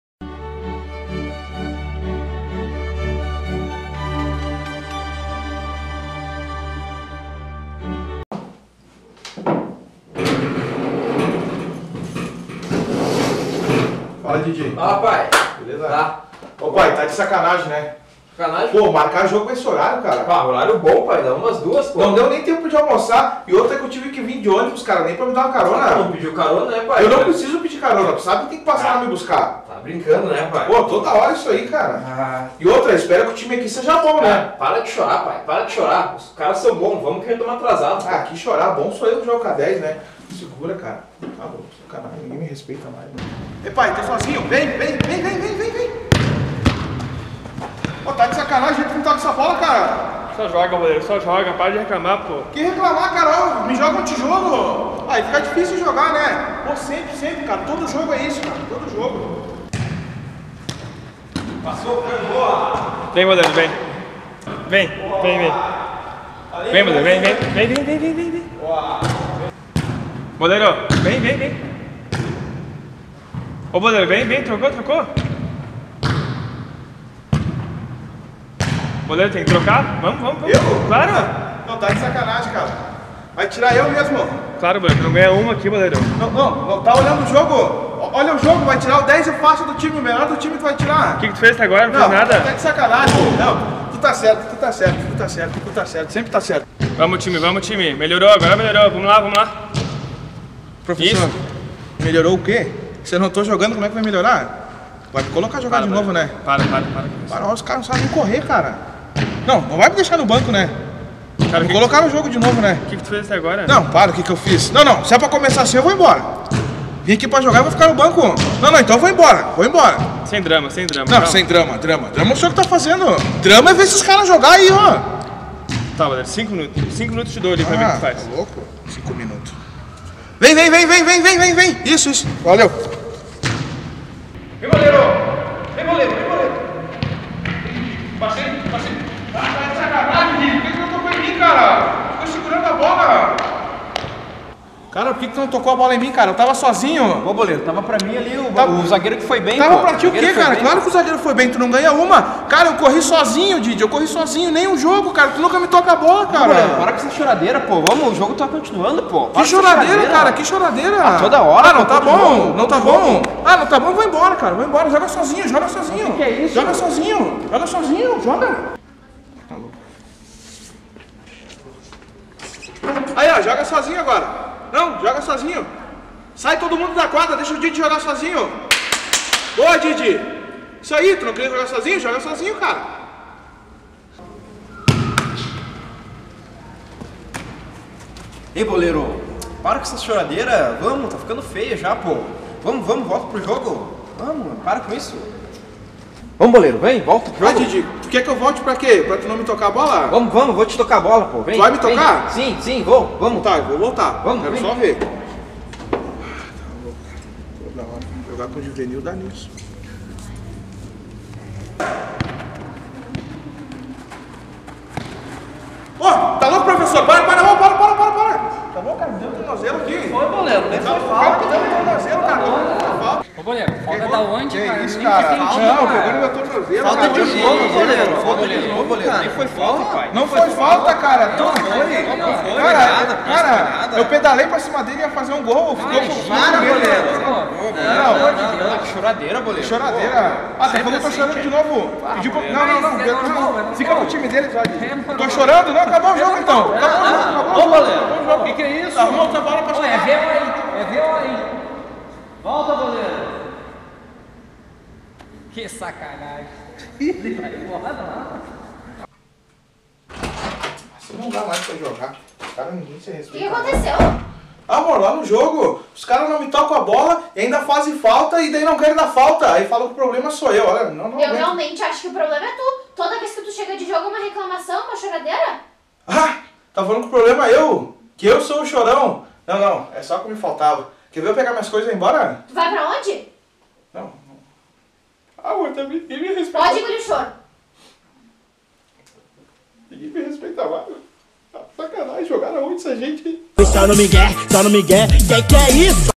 BOLEIRO CHORÃO
Tem muito Boleiro que quer meter passada mas só sabe chorar.

Meu Insta: @oboleiro_10
Meu Twitter: @oboleiro10